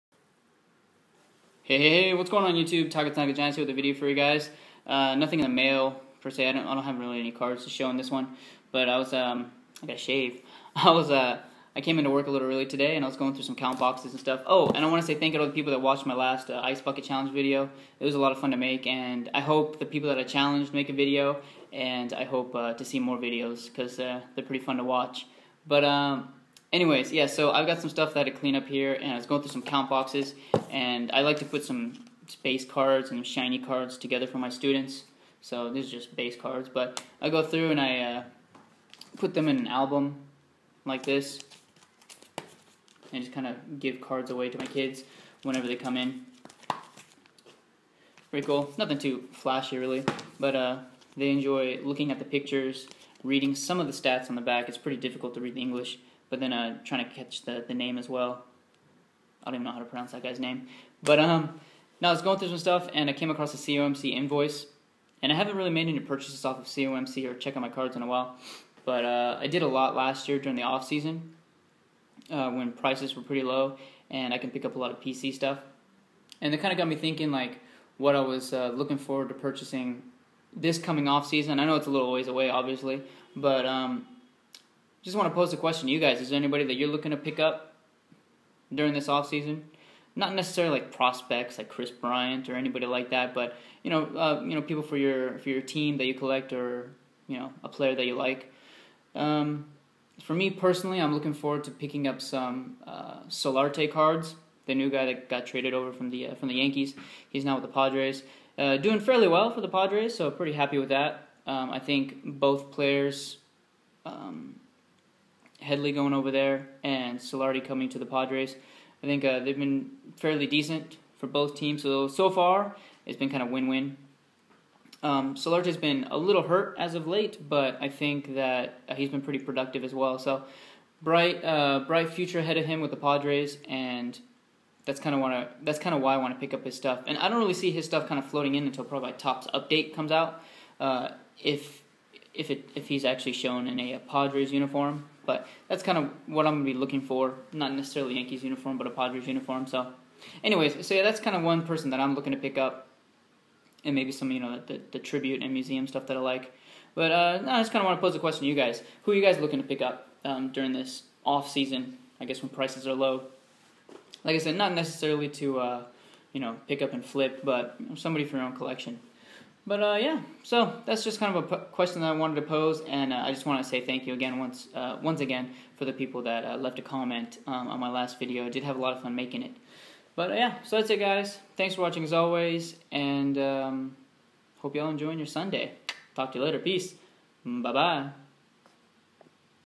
Off-season pickups. Who Are You After?
The off-season is a little ways away, but I'm excited about cheap prices :-)
Just a quick question to you folks...who/what are you looking to pick up on the cheap?
I'll be hunting for Solarte in a Padres uni.

Happy Collecting my friends :)